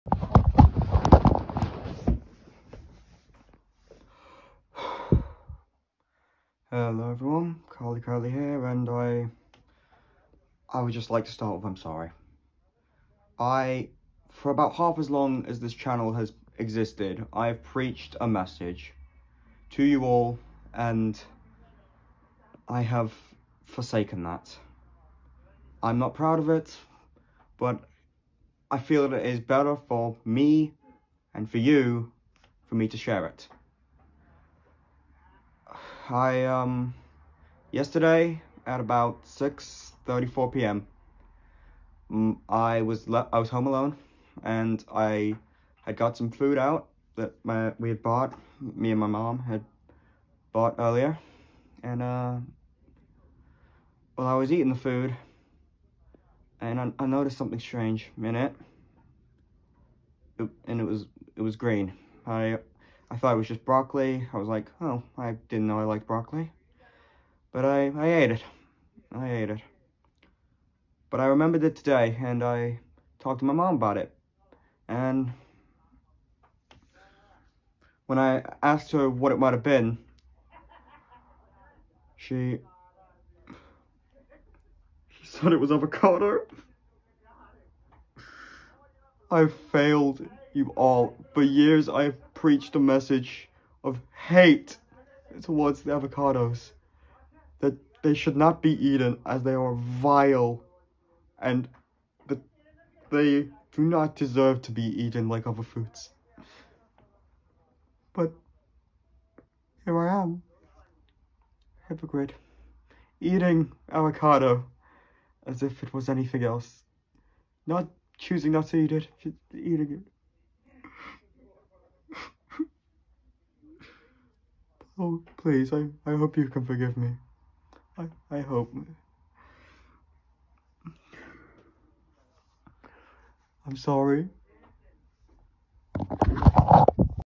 I’m Sorry…
Guys, once again, I would just like to say I’m sorry. I know it doesn’t mean anything, but I didn’t know it was avocado as I was eating it. I know, I should’ve checked the ingredients, & gotten rid of it immediately, but I was reckless, & instead chose to eat it. I hope you can all forgive me…